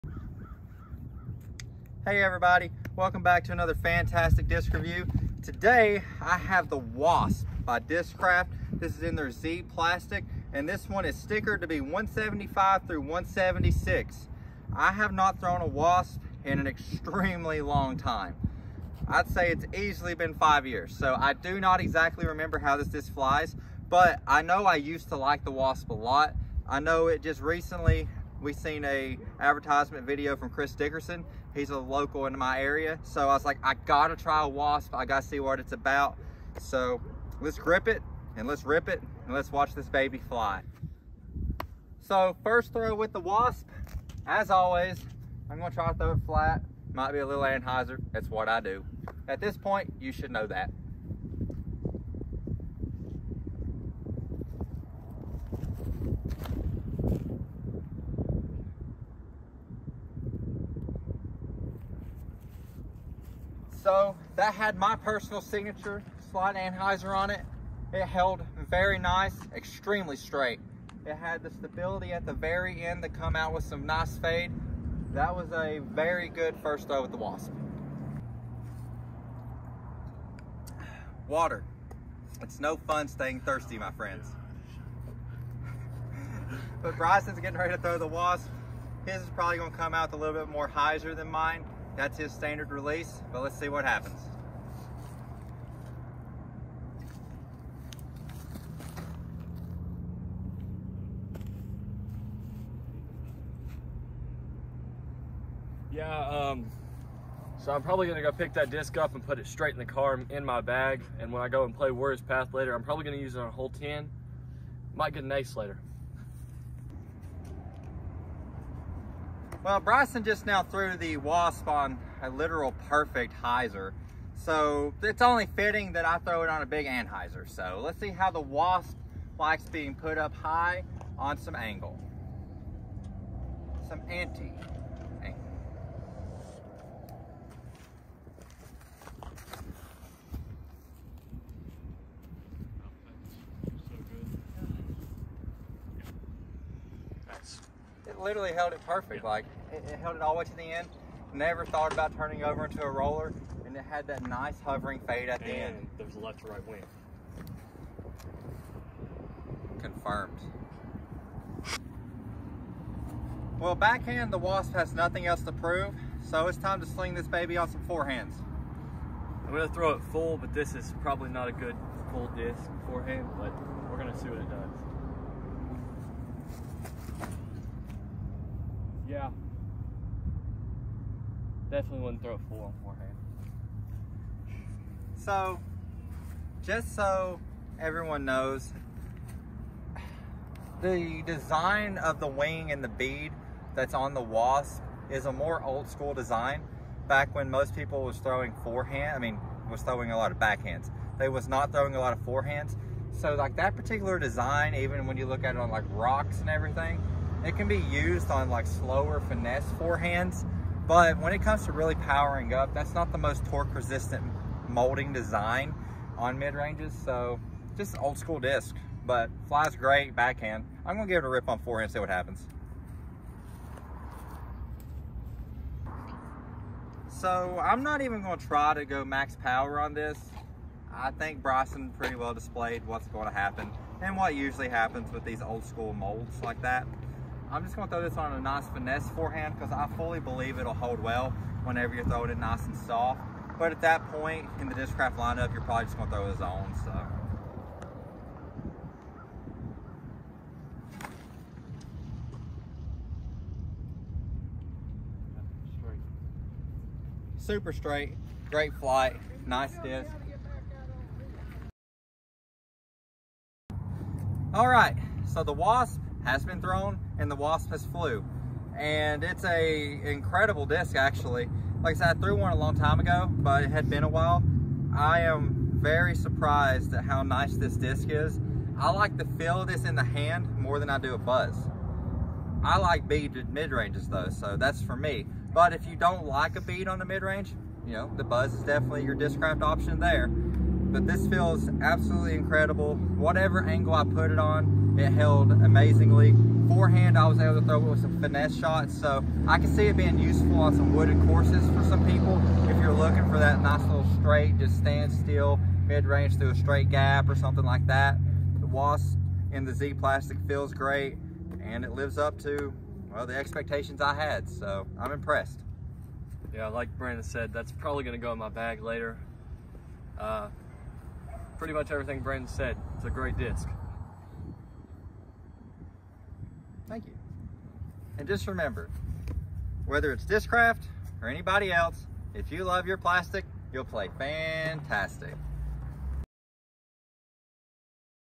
Put It Straight In The Car???? Discraft Wasp Review!
Discraft Wasp
Factory Flight # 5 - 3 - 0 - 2
Reviewed # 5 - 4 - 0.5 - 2